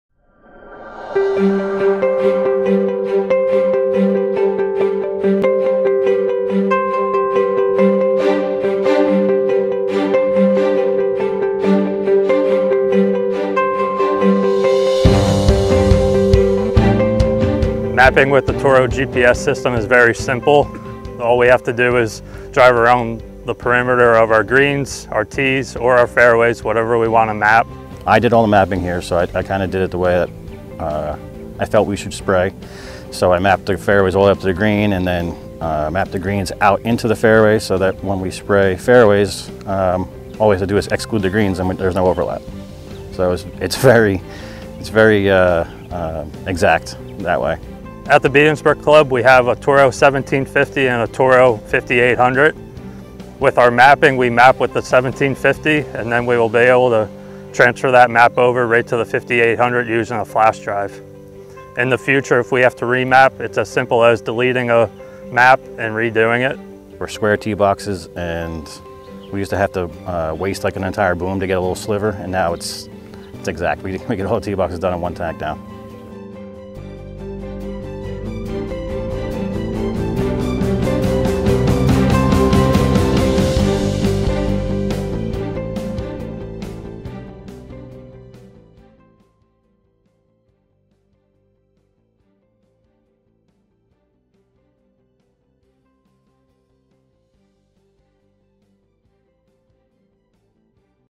Toro GeoLink: Mapping Solutions
The all-new GeoLink™ is the industry’s first turf-based precision spray system. Turf professionals will benefit from substantial chemical savings thanks to individual nozzle control and the available sub-inch accuracy. Reporting is simple with GeoLink; application rates, coverage maps, area and volume data are easily downloaded for thorough record keeping. The weatherproof touchscreen is rugged enough to withstand the most challenging conditions and makes application programming straightforward with just the touch of a button.